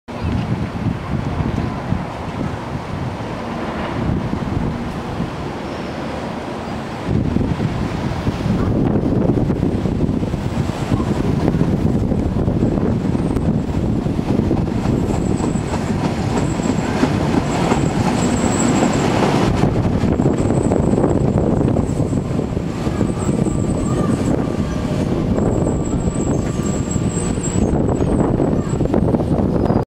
Tyne and Wear Metro - Metrocars 4077 and 40XX enter Northumberland Park
Metrocars 4077 and 40XX enter Northumberland Park for South Shields.